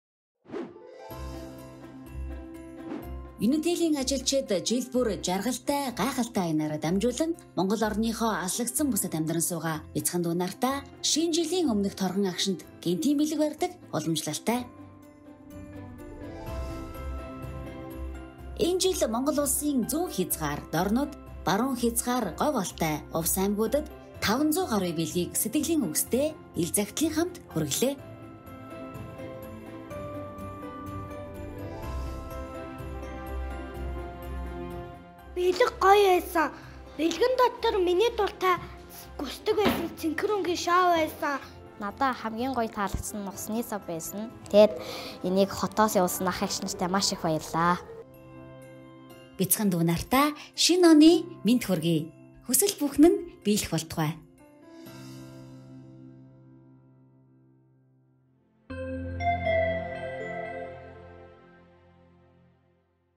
Жаргалтай гайхалтай | Unitel
Ногоон гэр бүлийн гишүүдийн санаачлан 4 дэх жилдээ хэрэгжүүлж буй "Жаргалтай гайхалтай" аянаараа дамжуулан алс хол суугаа дүү нартаа бэлэг хүргүүлж, шинэ жилийн ид шид түгээлээ 💚